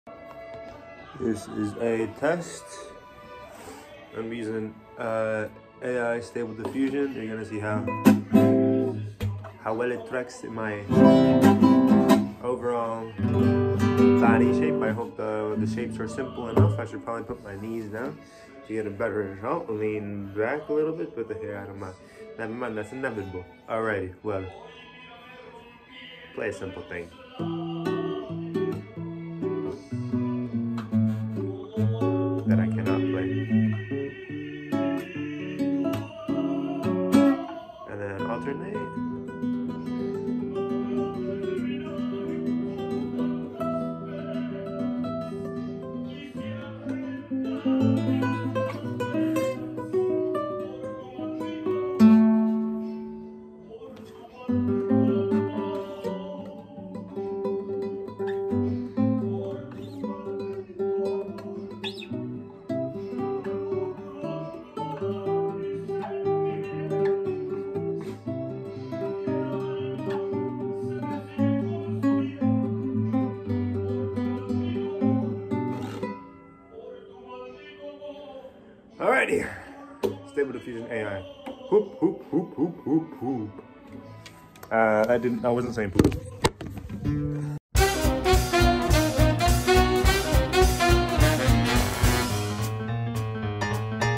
ai "stylizing" (but I can't specifically train my own ai) a guitar video
Stable Diffusion,
Nitrosocke is a ggwp kind of dude. the clutchinator if you will. A catalyst for the future.

Cannot train my own ai through dreambooth (it simply just does not generate, I've tried) because I only have 8GB of VRAM, when you need 12 to 13 to properly operate.

Update: I have discovered Lora, have not watched the video, but I discovered it, (it lets you train models with less than 7GB of VRAM... AND I HAVE 8. Ah yes, mankind, personkind, artificialintelegencekind.

Still a fun little thing to do though, I should've ticked restore faces, but it might've looked goofier.
Only had one pass, did not deflicker nor put a directive motive behind the video other than to expirement, but maybe in the near future there will be a stylized mini movie.

Corridor crew is cool.
but not cooler than nitrosocke.

Link for the diffusion ckpt I used

First and only tutorial I watched a couple months back when I first did stable diffusion (if you're interested)

(watch one piece, you know who you are [ this is not targeted at the general public])